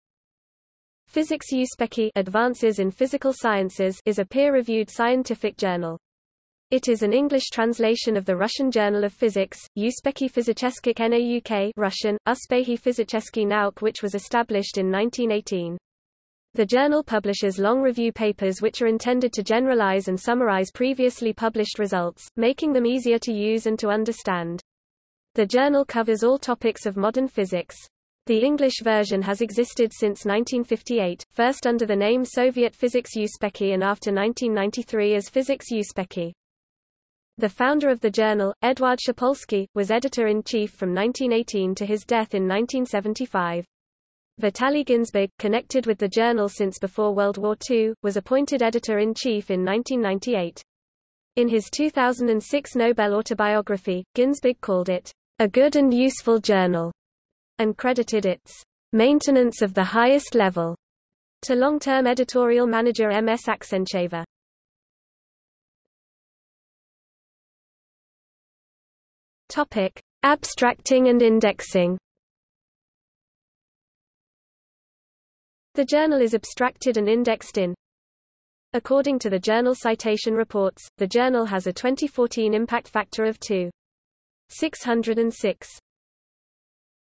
Soviet Physics Uspekhi | Wikipedia audio article
00:01:05 Abstracting and indexing

- Socrates

SUMMARY
Physics-Uspekhi (Advances in Physical Sciences) is a peer-reviewed scientific journal. It is an English translation of the Russian journal of physics, Uspekhi Fizicheskikh Nauk (Russian: Успехи физических наук) which was established in 1918. The journal publishes long review papers which are intended to generalize and summarize previously published results, making them easier to use and to understand. The journal covers all topics of modern physics. The English version has existed since 1958, first under the name Soviet Physics Uspekhi and after 1993 as Physics-Uspekhi.
The founder of the journal, Eduard Shpolsky, was editor-in-chief from 1918 to his death in 1975. Vitaly Ginzburg, connected with the journal since before World War II, was appointed editor-in-chief in 1998. In his 2006 Nobel autobiography, Ginzburg called it "a good and useful journal" and credited its "maintenance of the highest level" to long-term editorial manager M. S. Aksentyeva.